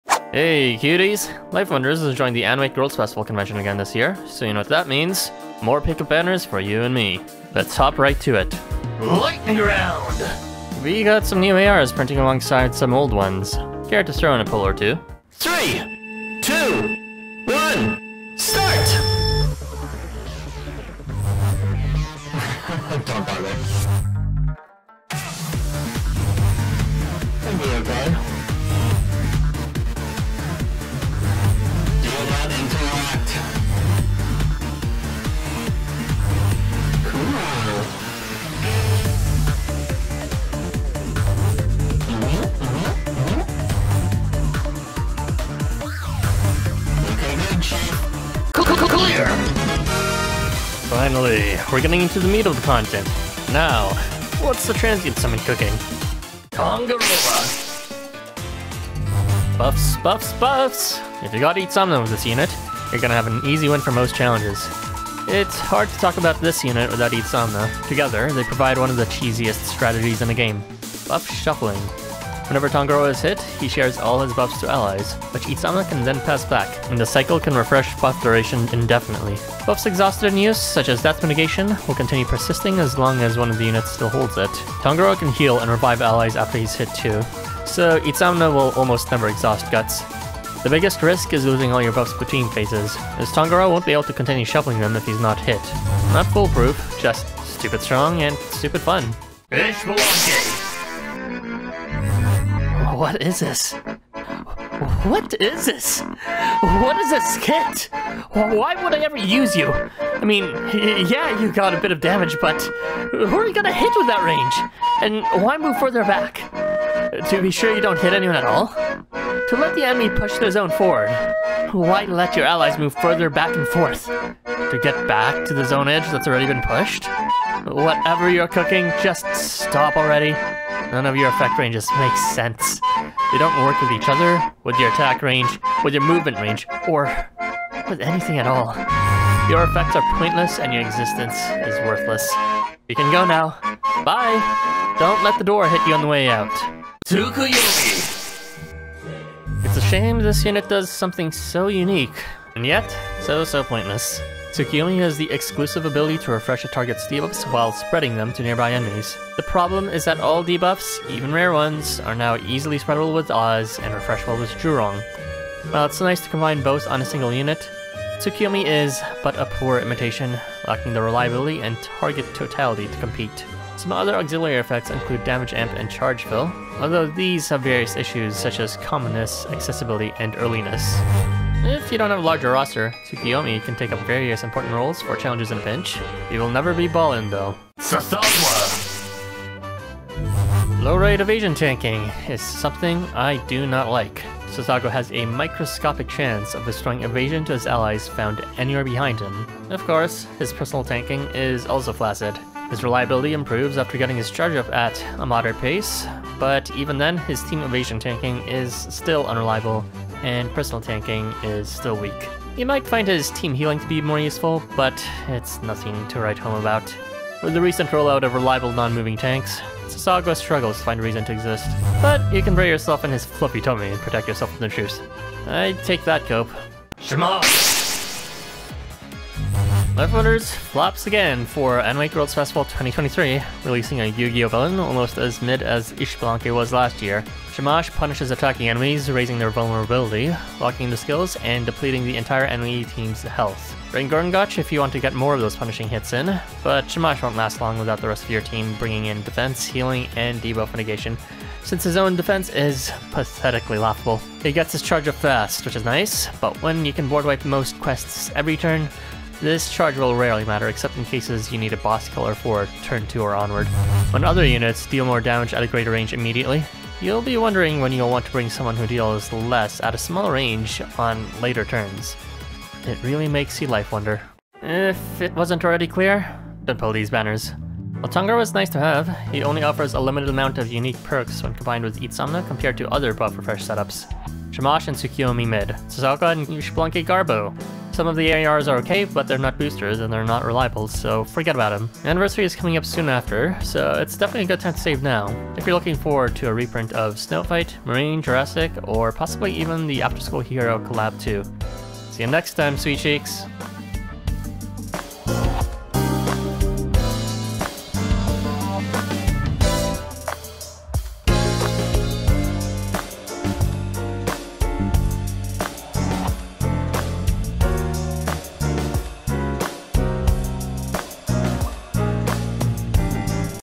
Should You Pull On The Bishie Banner? 🤔
0:00 Start!

0:08 ⚡ LIGHTNING ROUND ⚡ AGF AR Summon

AGF Transient Summon
0:56 Tangaroa [Canaan]
1:39 Ixbalanque [Rare]
2:27 Tsukuyomi [Night Festival]
3:08 Tsathoggua [Fashionista]
3:47 Shamash [Rare]

4:34 Should you pull?

5:04 Support and resources